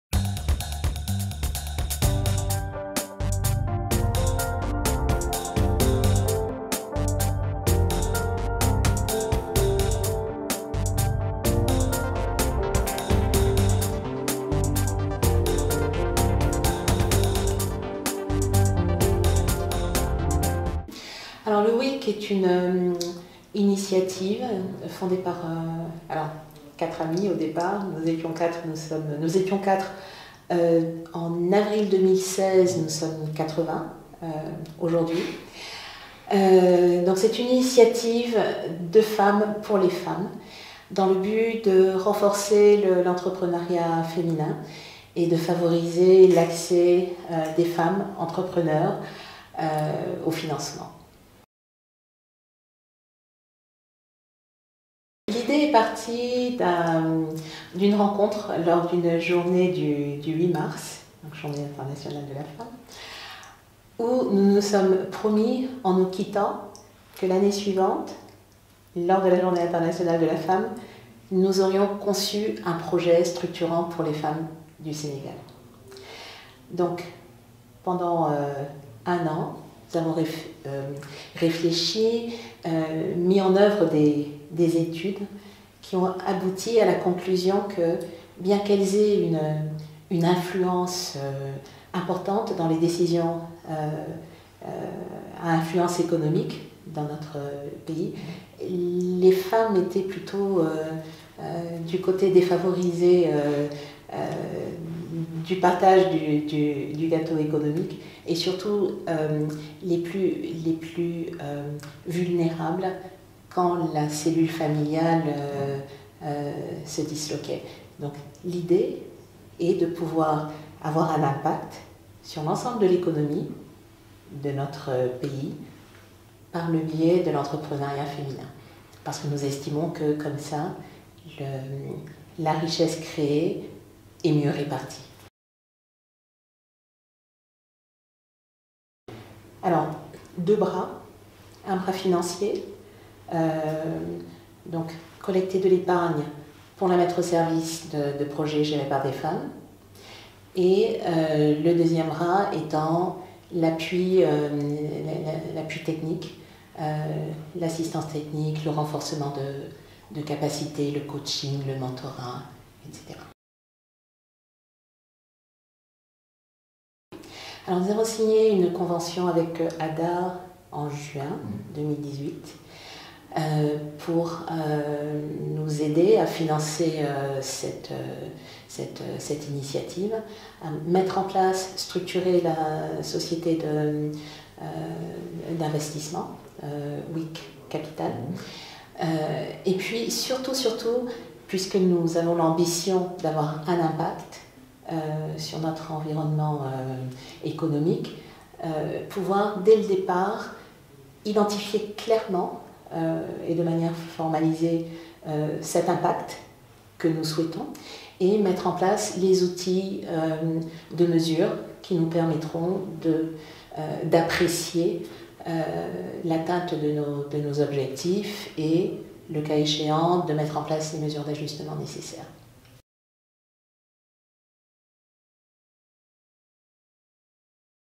Interview de Thiaba Camara Sy, co-fondatrice du Women’s Investment Club Sénégal (WIC)
Une interview de Thiaba Camara Sy, co-fondatrice du Women’s Investment Club Sénégal (WIC)

Le WIC vise à renforcer la présence des femmes sur les marchés financiers ainsi qu’à améliorer l’accès au financement et le soutien technique aux femmes entrepreneurs porteuses de projets prometteurs. Depuis sa création en 2016, le club regroupe 78 femmes avec un capital collectif d’investissement investi au niveau de la Bourse Régionale des Valeurs Mobilières (BRVM). Le club a tenu un grand nombre d’activités pour améliorer la compréhension du marché financier par ses membres de même que pour identifier et accompagner des femmes entrepreneurs au Sénégal. WIC entend élargir son impact dans l’écosystème entrepreneurial féminin en nouant des partenariats avec des structures comme ADA qui ont au cœur de leurs activités le déploiement innovant de capital financier pour un impact social largement ressenti au sein des populations.